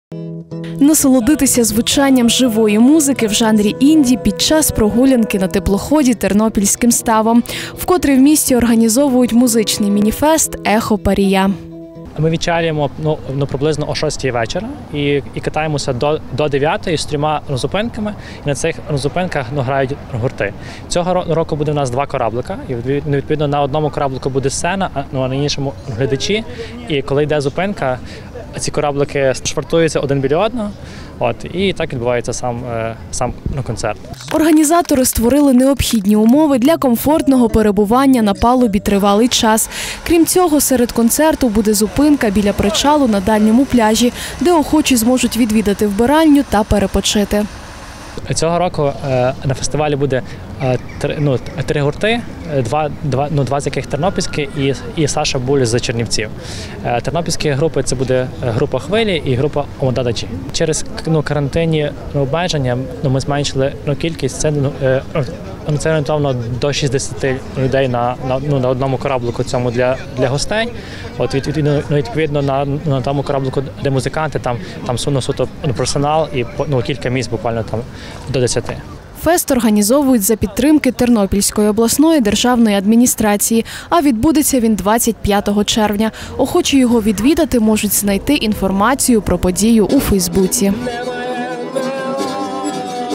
Музичний міні-фест "Ехо Парія" відбудеться у Тернополі 25 червня
Музичний міні-фест "Ехо Парія" відбудеться у Тернополі 25 червня. Цього року відвідувачі заходу зможуть насолодитися живим виконанням місцевих гуртів і запрошеного гостя.